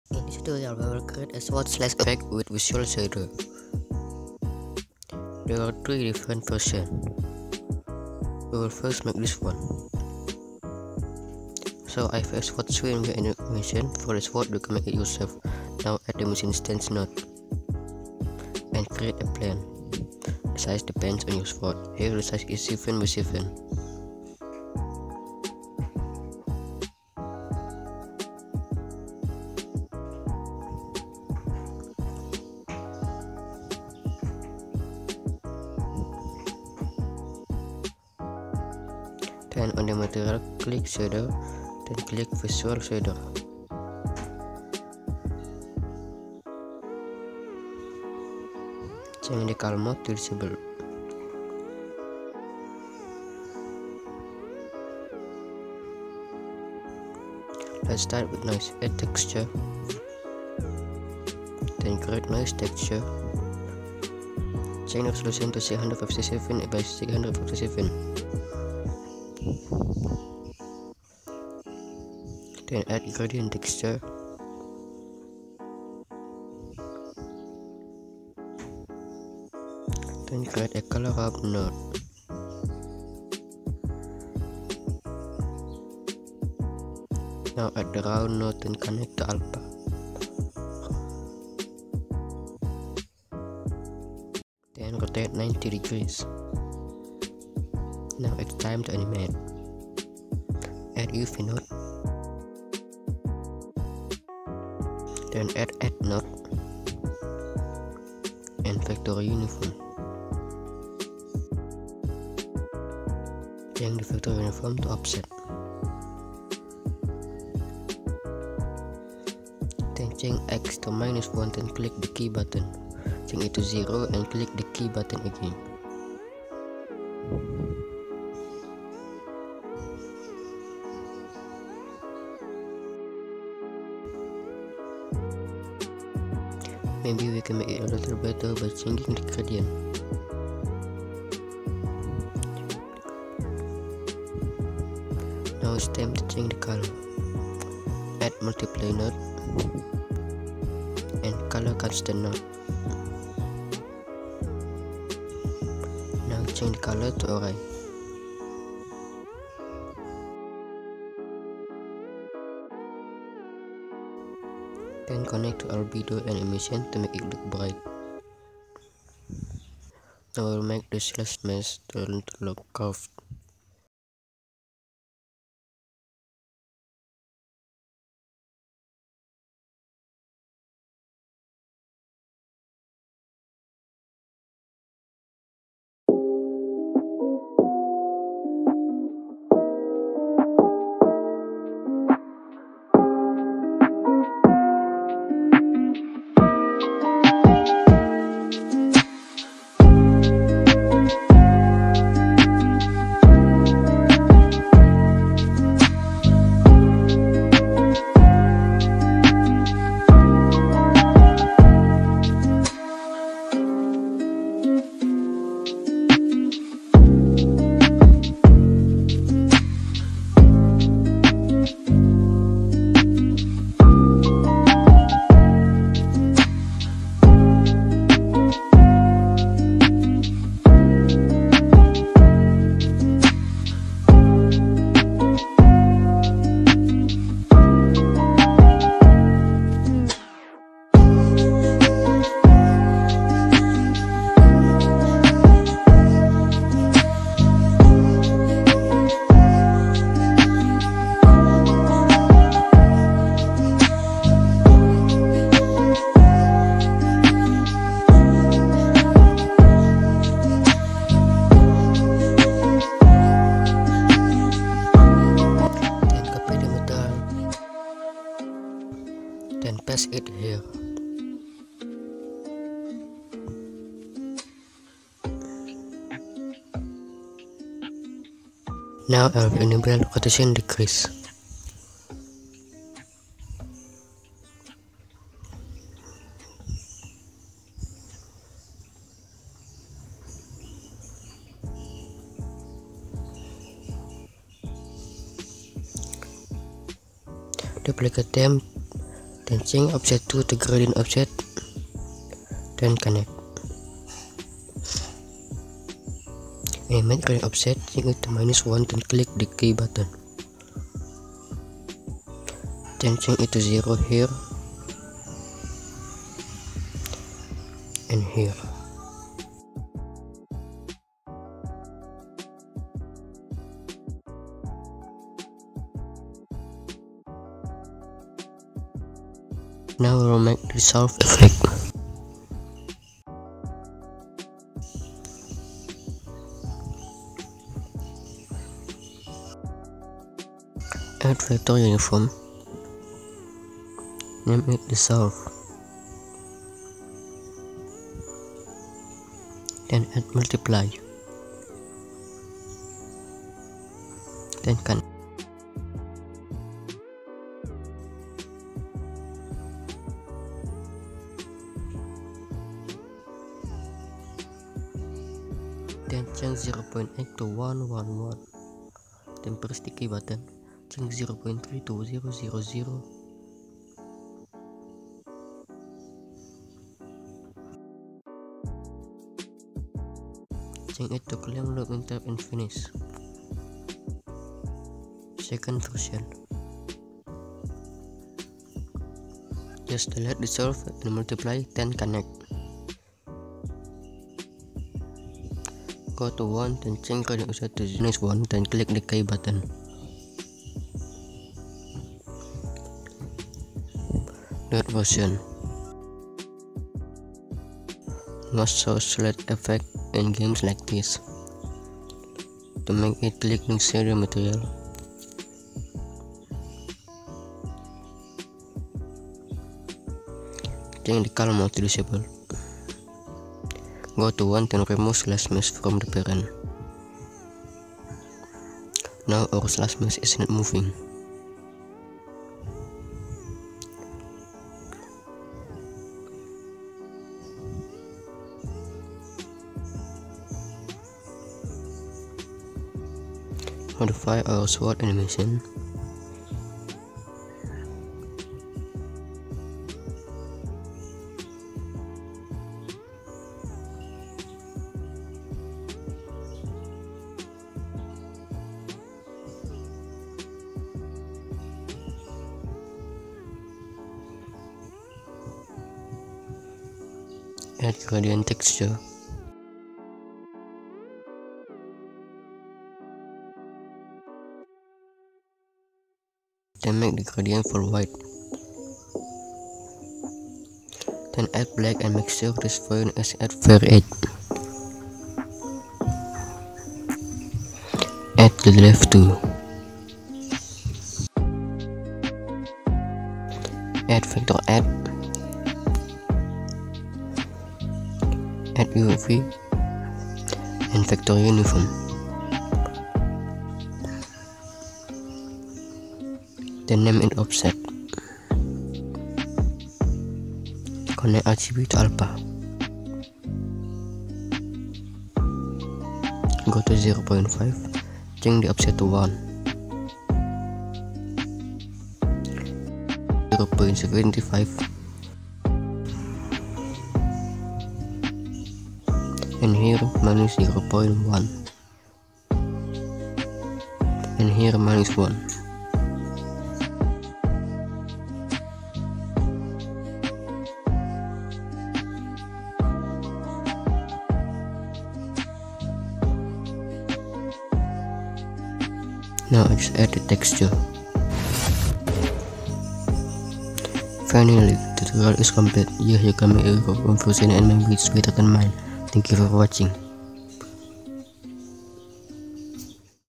Godot Tutorial - How to make Sword Slash Effect using Visual Shader - 3 different versions
How to make a Sword Slash or Sword Swing effect on Godot using Visual Shader
Works on GLES3 and GLES2 (I just tested it on GLES2 and it's cooler!)
There are 3 different versions

Timestamps:
00:09 = Version 1
02:54 = Making Slash Mesh (so the effect looks curved)
06:26 = Version 2 (You must make version 1 too)
06:46 = Version 3

Facebook - More and detailed updates are there! (Yeah previously it's a fake account)
Twitter (I rarely post tutorials there)